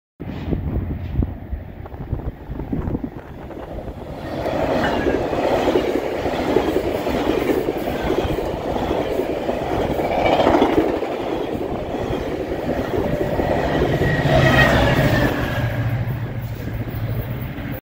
GO 1873 (Hamilton Express) - 371 With 618 At Mimico
Destination: Hamilton GO Center

(Express From Union To Clarkson)